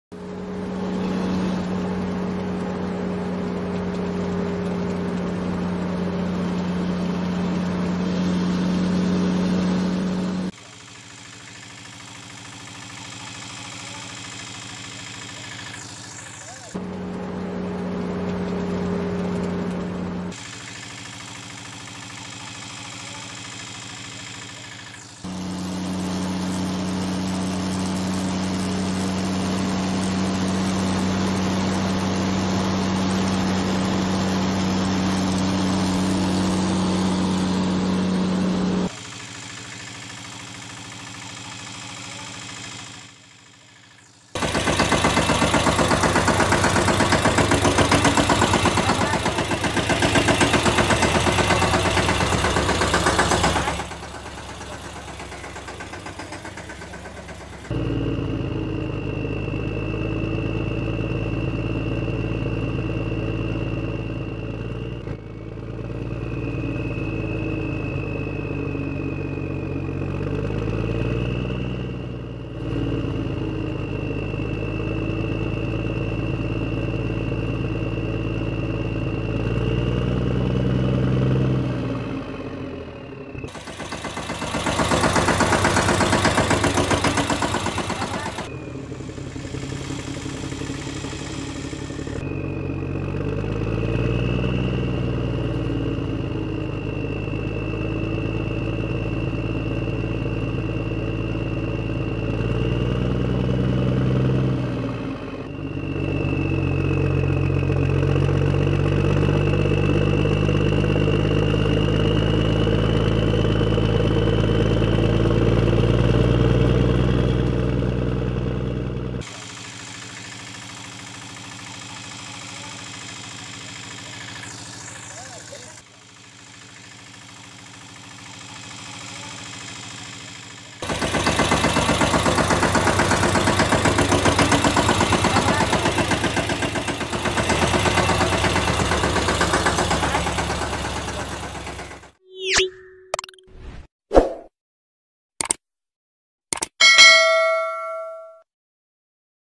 Truck Got Stuck While Carrying Sand, Then It Was Pulled Out With The Help Of JCB
The truck was carrying sand, when suddenly it got stuck in the sand, then it was pulled out of the sand with the help of JCB